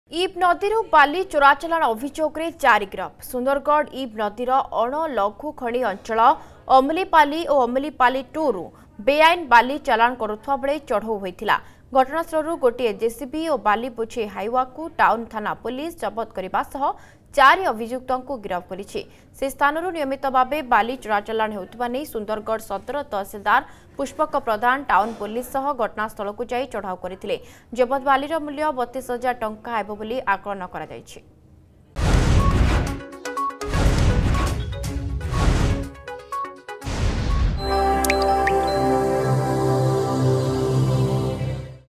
Illegal Sand Lifting from IB River Bed, 4 Arrested
Illegal Sand Lifting from IB River Bed in Sundargarh district, 4 Arrested.